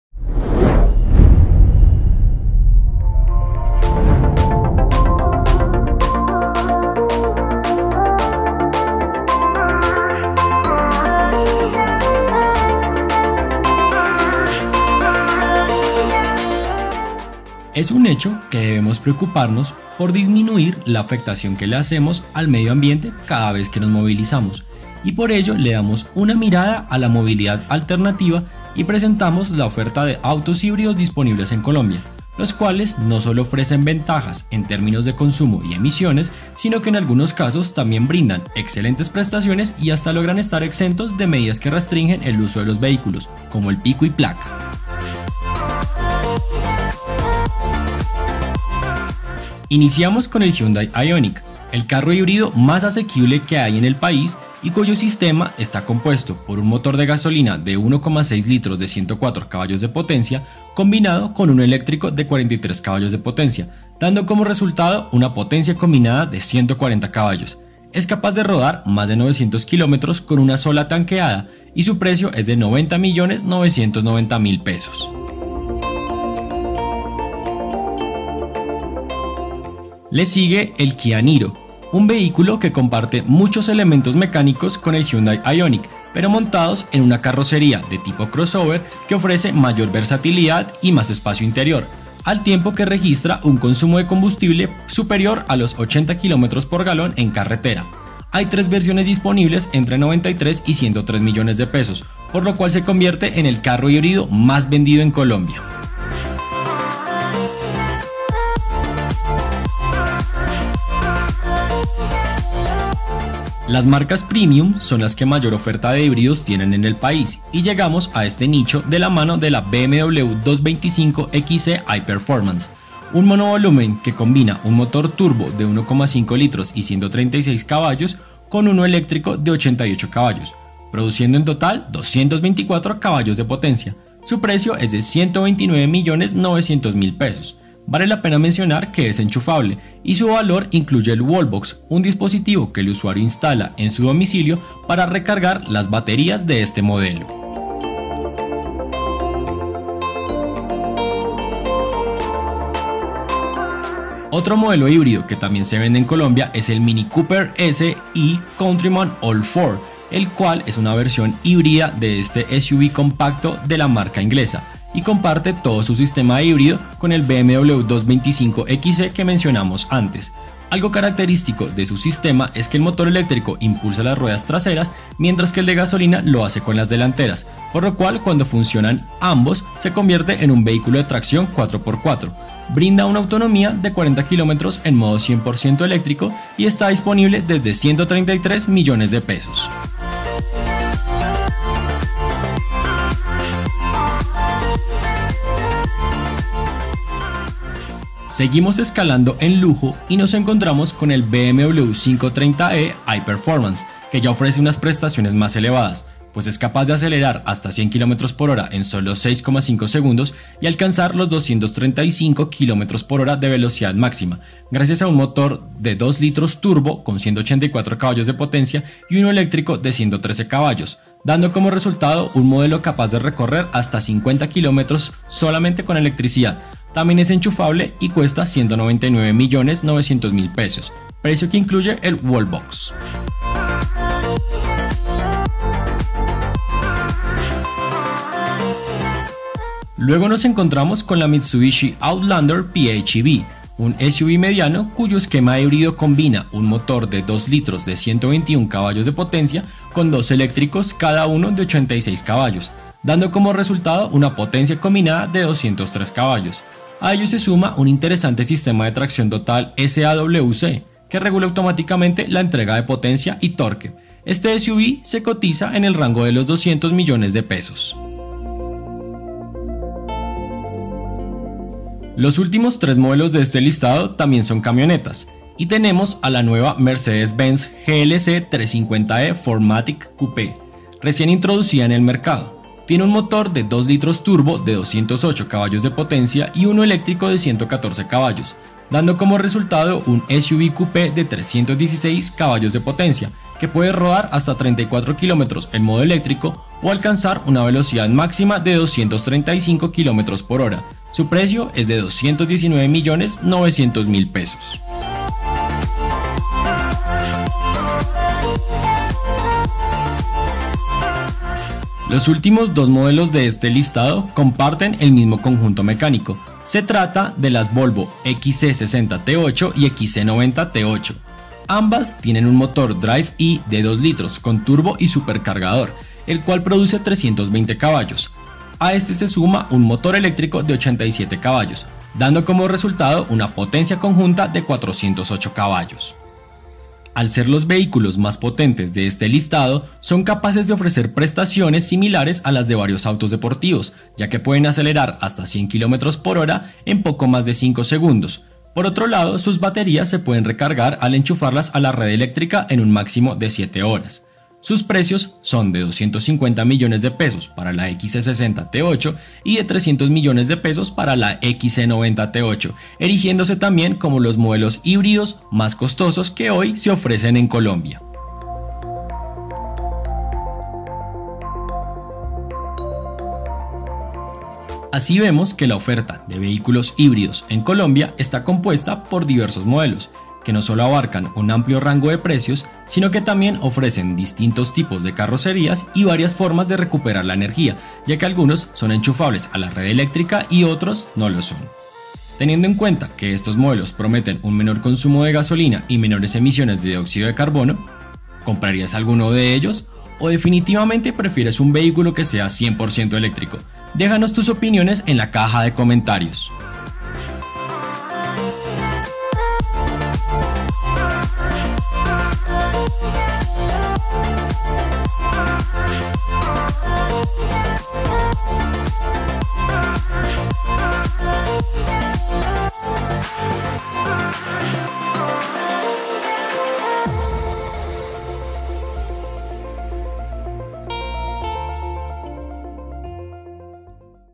Carros híbridos en vitrina ⚡¿Cuál te comprarías? ⚡
Les presentamos el listado de los carros híbridos que actualmente se venden en Colombia, ya sean HEV (normal = Hybrid Electric Vehicle) o PHEV (enchufable = Plug-in Hybrid Electric Vehicle). ¿Cuál te gusta más?